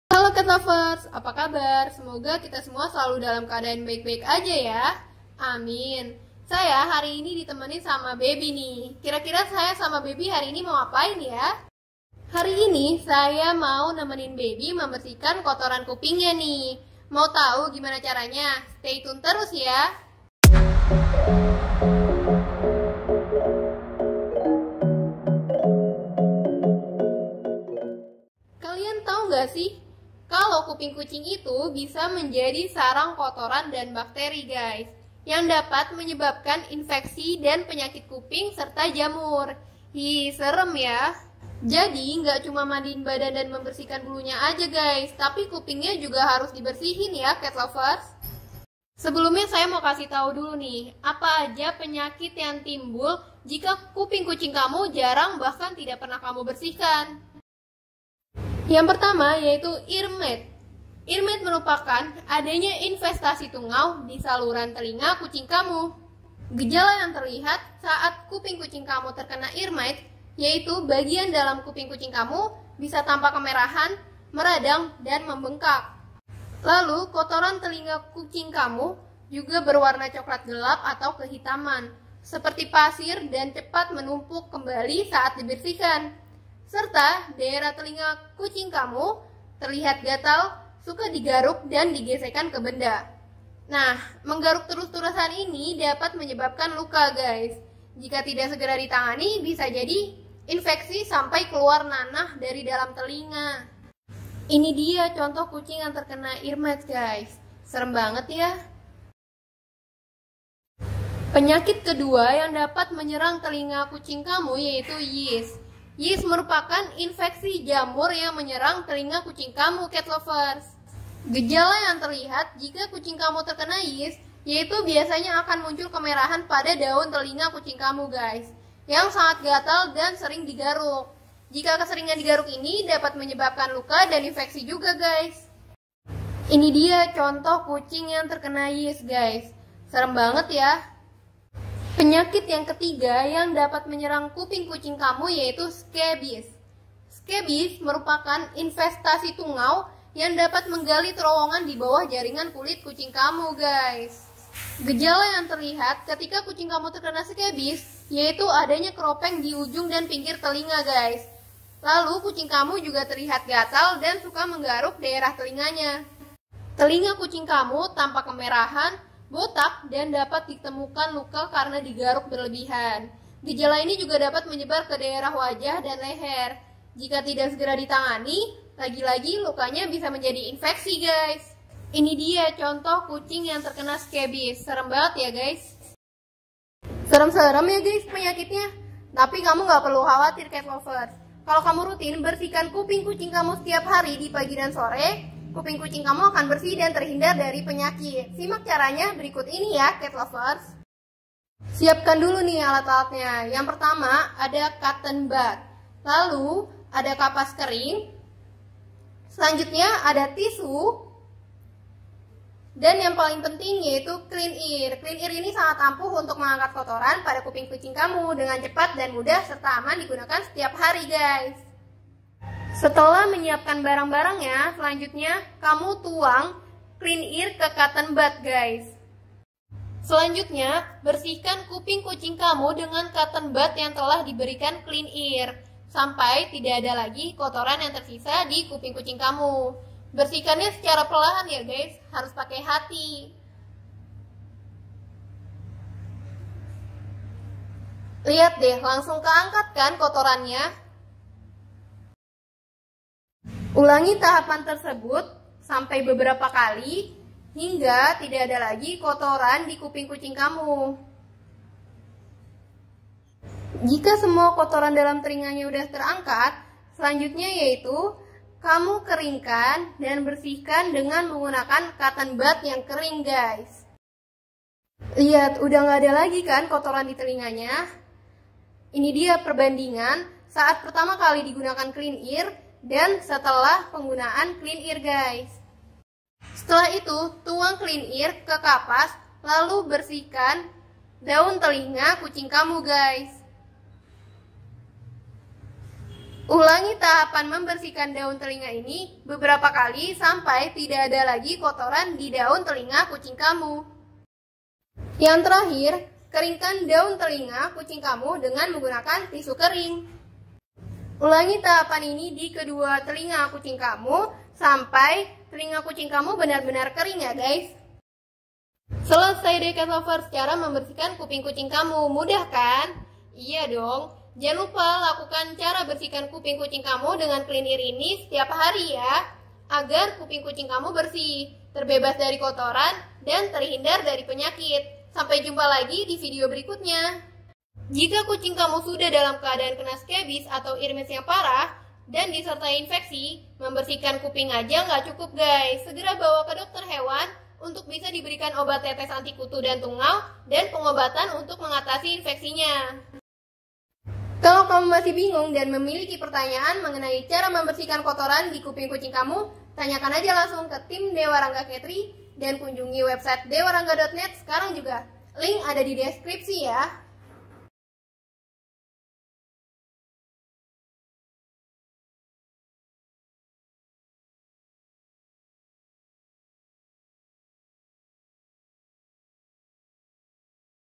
((MUST WATCH)) Cara Mudah Bersihkan Kuping Kucing
Kamu suka perhatikan gak sih kuping kucing Kamu?

Gak pernah yaa?

Kuping kucing Kamu wajib diperhatikan guys, karena kuping kucing Kamu bisa jadi sarang bakteri dan penyakit!

Cari tau caranya di video ini, tonton sampai habis ya!

Beli KLIN EAR dan kunjungi dewarangga.net sekarang juga!